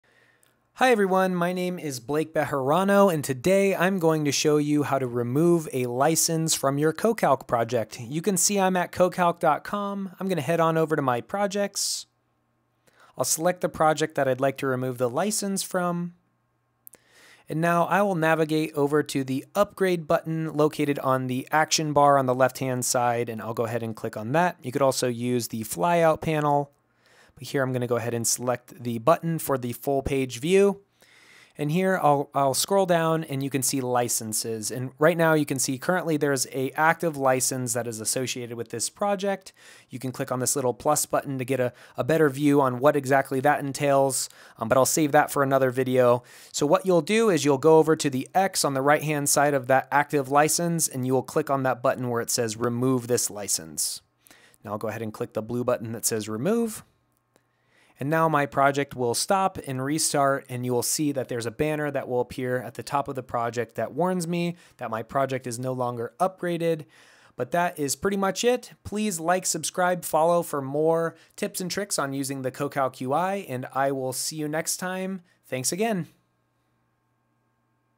How to Remove a License from your CoCalc Project
In this video, we’ll demonstrate how to remove a license from your CoCalc project. You will learn how to:

- Navigate to the Upgrade Button within your CoCalc project
- Remove an assigned license from your project
- Confirm the removal of the license and its implications

Removing a license is beneficial for:

- Managing your resource allocations efficiently
- Allocating licenses to other projects or users as needed

Join us and discover how to manage your CoCalc project's licenses effectively by removing them when they are no longer needed!

Navigate to the Upgrade Button:
Open your project and navigate to the upgrade button on the action bar (located on the left-hand side).

Within the Upgrade Panel, find the list of active licenses associated with your project.

Select the license you wish to remove and click on the Remove button next to it.

Confirm any prompts to ensure that the license is removed correctly.

Confirm the Removal:

Ensure that the license is no longer listed in your project's licenses section.
Understand that removing a license might affect certain features or access levels within the project.

Enhance your resource management by effectively handling your project's licenses with ease!